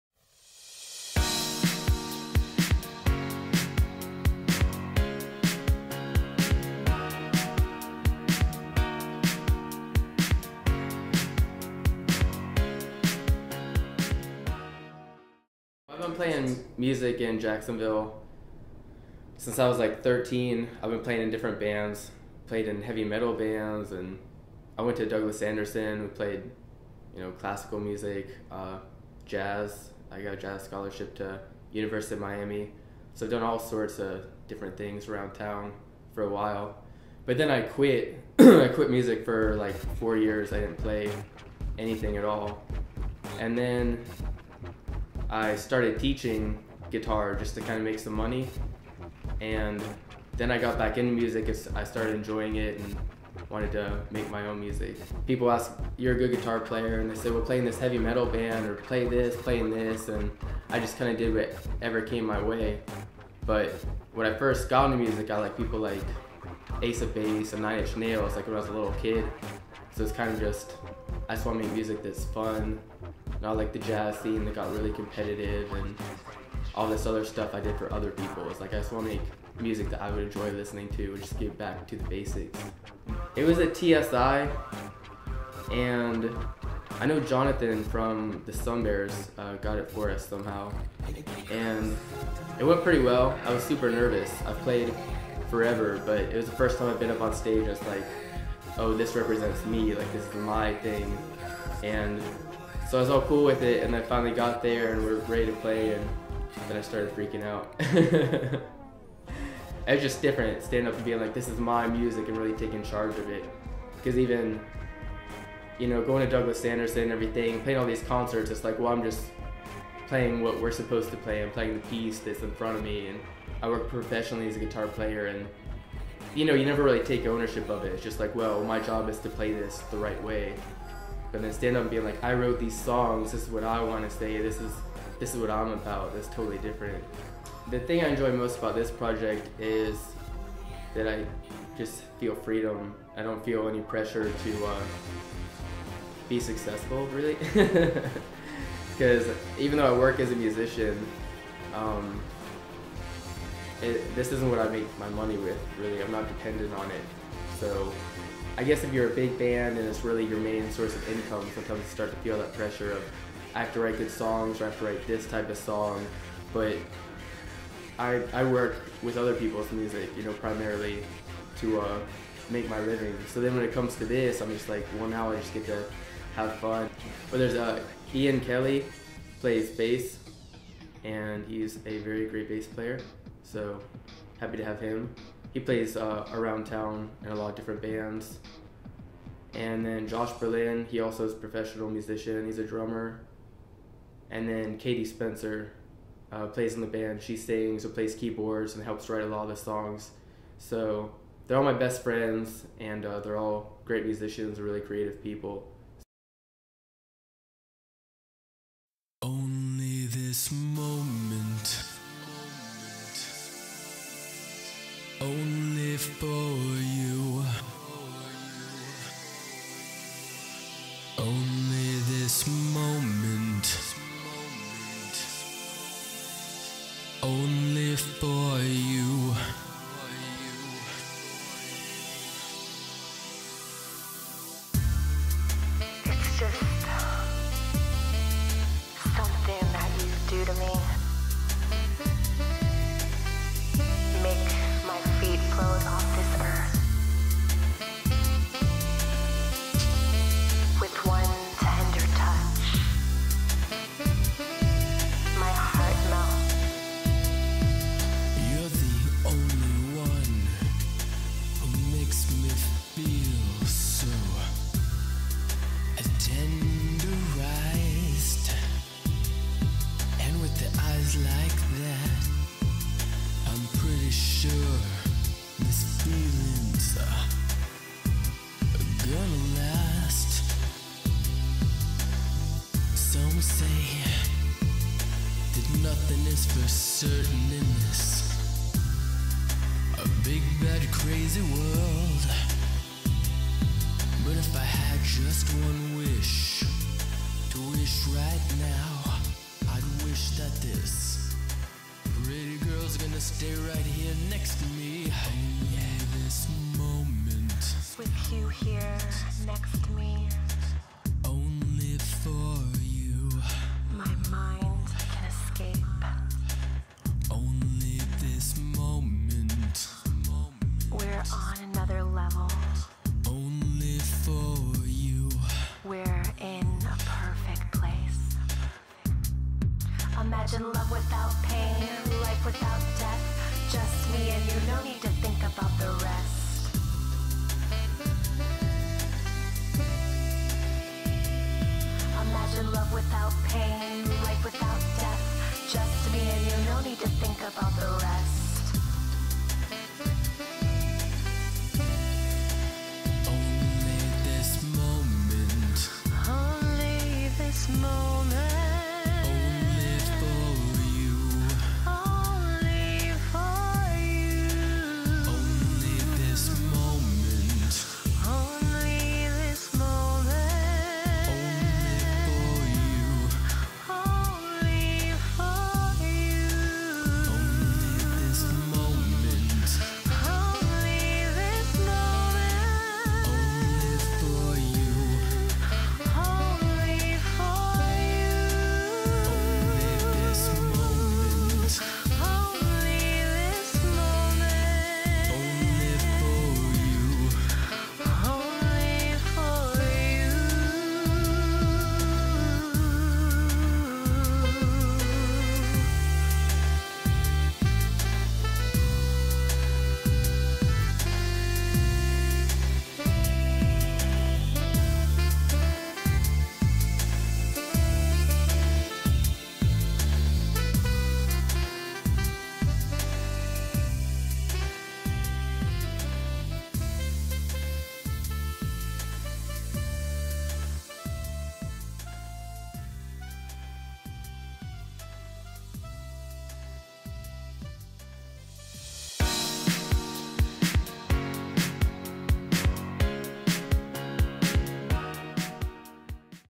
Juicy Pony
Jesse Leach is the front man/producer behind the Jacksonville-based electronic-rock band, Juicy Pony.